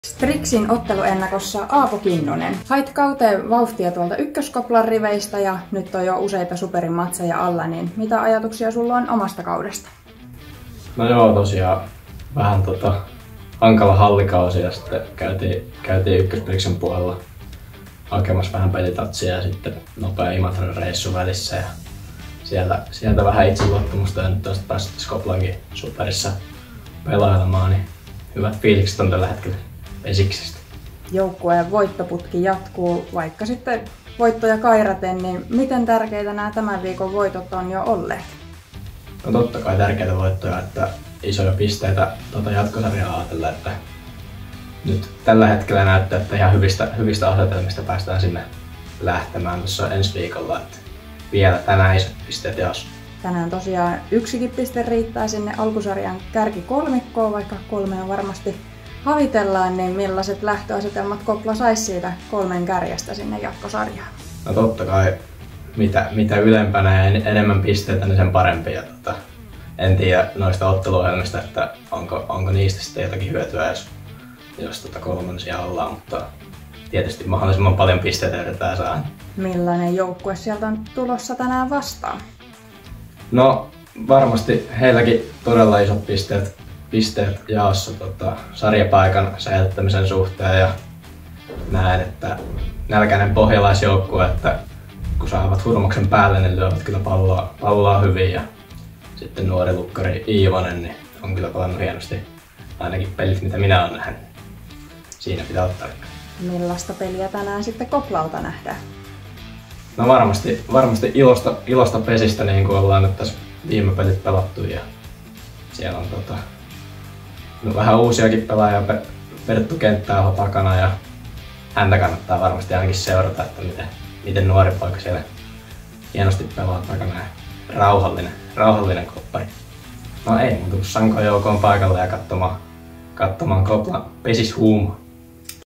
Strixin otteluennakko KPL - AA 28.7.2024, haastattelussa Aapo Kinnunen
Alkusarjan viimeinen ottelu pelataan KSS Energia Areenalla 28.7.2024 ja vastakkain KPL - AA.

Kausi jatkuu ylemmässä jatkosarjassa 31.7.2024 alkaen.

Strixin otteluennakossa Aapo Kinnunen.